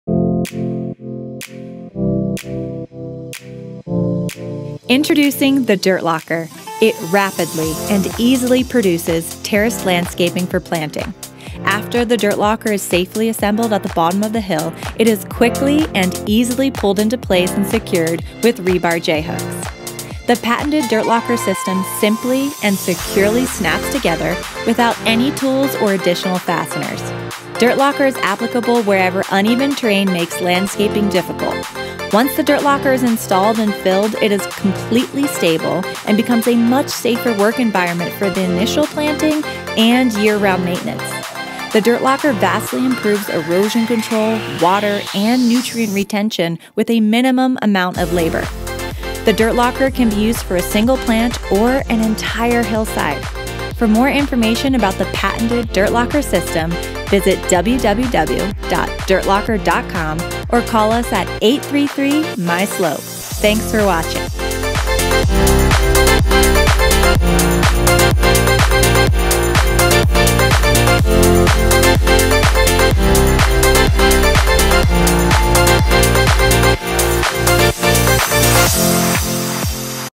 Dirt Locker® - the Hillside Terracing and Erosion Control System Intro
Introducing the Dirt Locker® Erosion Control Product for simple landscaping on sloped backyards. Landscape your steep slope or hillside for low maintenance. Protect your hillside from soil and water erosion. No need to build expensive retaining walls.

Dirt Locker® is a sloped landscaping solution for hillside front yards and backyards that experience erosion and irrigation issues. We're on a mission to solve your sloped yard's engineering issues with our simple product! The Dirt Locker® gets your garden dirt locked and ready to load with fresh fruits, vegetables and plantings in a matter of hours. Learn more about how Dirt Locker® offers hillside transformations from a variety of steepness levels on our FAQ page (link above). Thank you for supporting Dirt Locker®!